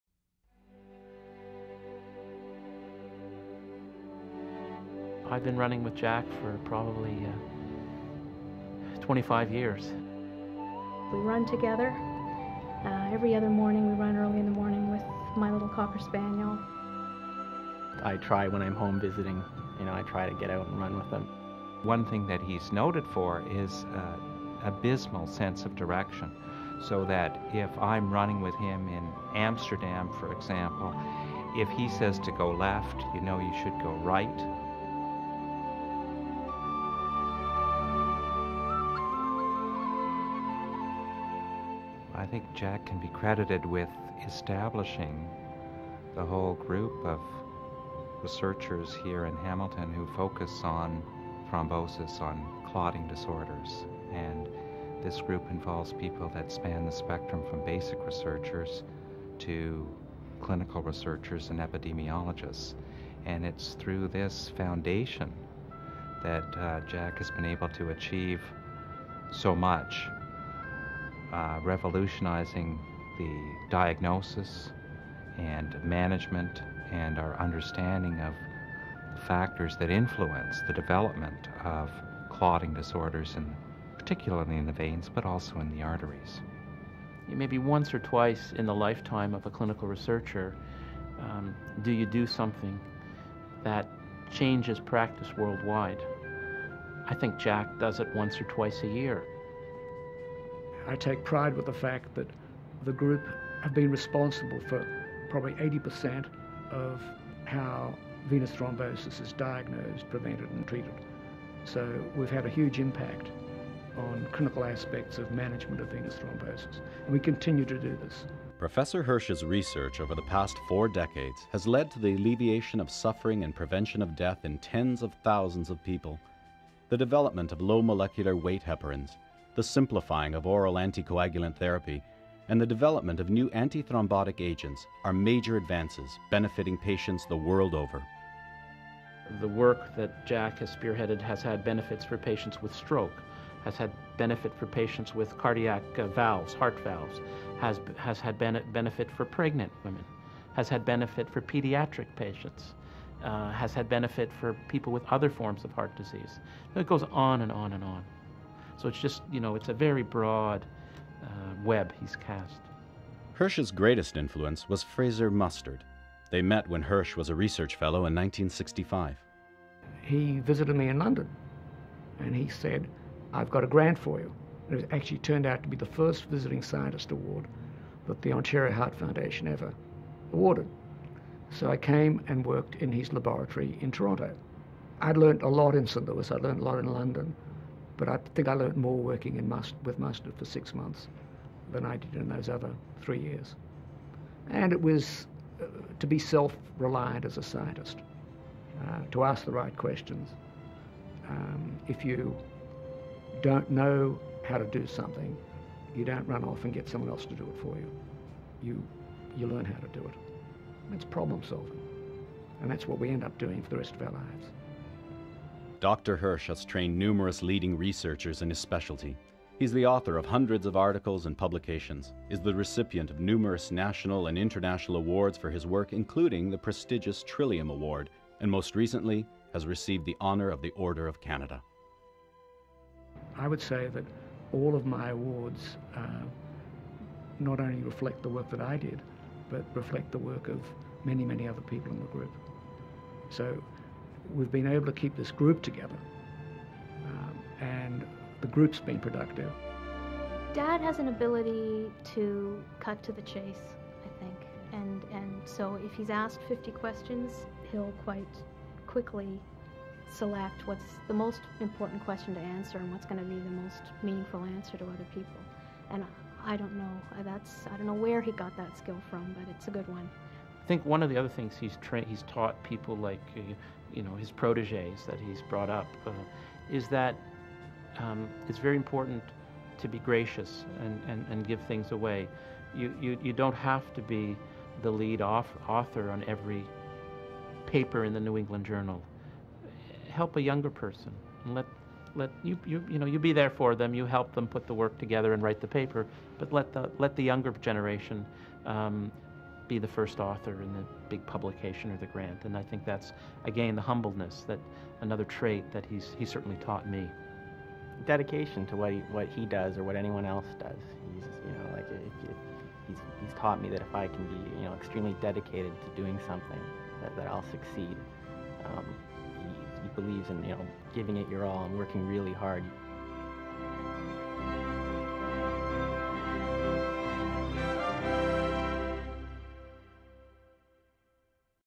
Dr. Jack Hirsh Canadian Medical Hall of Fame Laureate 2000
Dr. Hirsh has combined an active laboratory search for new and more effective anti-clotting drugs (low molecular-weight heparins) with a wide-ranging clinical study of their effectiveness. As the result of his studies, it has become possible to improve the treatment of deep-vein thrombosis, and to adjust optimally the dosage of oral anti-coagulant medications that reduce the chance of heart and stroke in persons at risk. As Director of the Hamilton Civic Hospital Research Centre at McMaster and as an active partner with several pharmaceutical firms, Dr. Hirsh has established the specialty of clinical thrombosis on a firm footing, and fostered a whole generation of young researchers in the field.